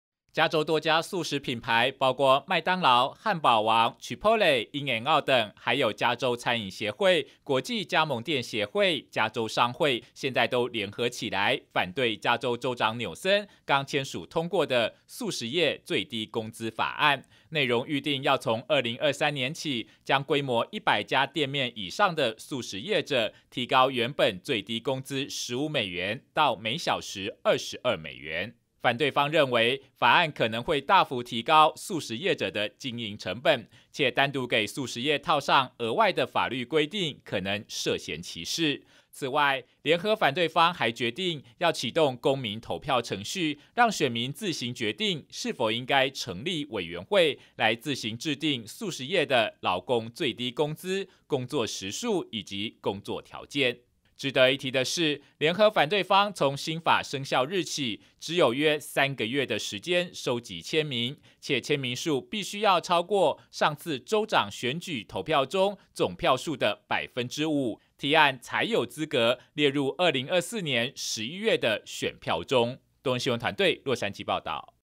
加州多家協會聯手 對抗"速食業最低工資"法/將推動全民公投 成立"速食業委員會"自治
加州多家速食連鎖店業者，現在決定聯合起來，啟動公民投票，目標要反對加州州長紐森，剛簽署通過的"速食業最低工資"法案。

加州多家速食品牌，包括麥當勞、漢堡王、Chipotle、In-N-Out等，還有加州餐飲協會、國際加盟店協會、加州商會，現在都聯合起來，反對加州州長紐森，剛簽署通過的"速食業最低工資"法案，內容預定要從2023年起，將規模一百家店面以上的速食業者，提高原本最低工資15美元，到每小時22美元。

反對方認為，法案可能會大幅提高，速食業者的經營成本，且單獨給速食業套上，額外的法律規範，可能涉嫌歧視；此外，聯合反對方還決定，要啟動"公民投票"程序，讓選民自行決定，是否應該成立委員會，來自行制定速食業的，勞工最低工資、工作時數，以及工作條件。

值得一提的是，聯合反對方從新法生效日起，只有約三個月的時間收集簽名，且簽名數必須要超過，上次州長選舉投票中，總票數的百分之5，提案才有資格，列入2024年11月的選票中。